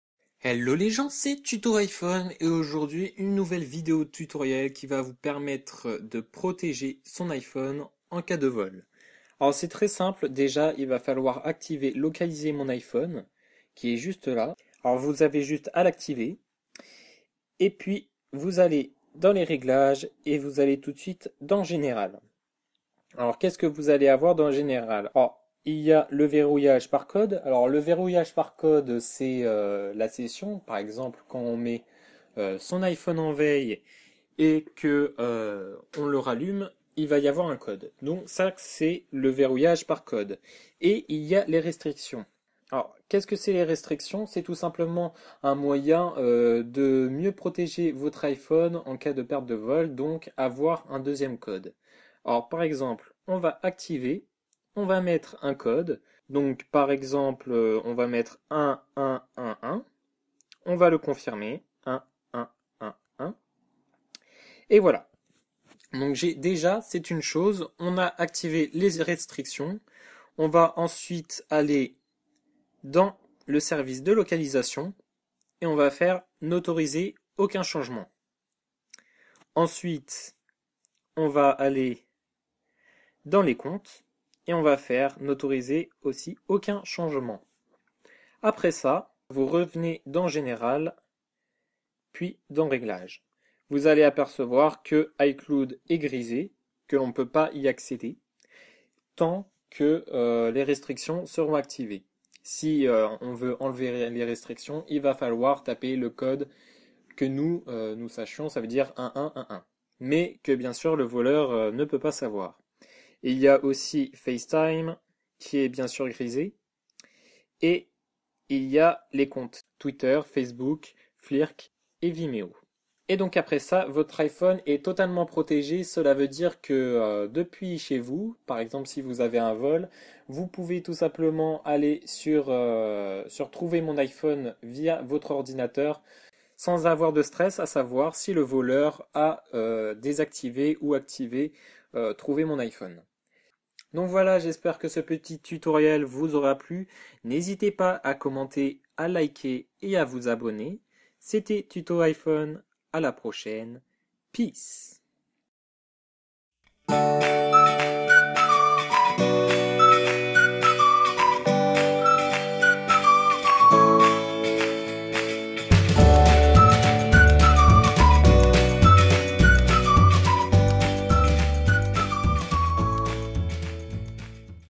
Proteger son iphone en cas de vol
Nouveau tutoriel pour vous expliquer comment sécuriser votre iphone en cas de vol
N’hésitez pas à commenter si vous ne comprenez pas quelque chose dans la vidéo

Tuto Iphone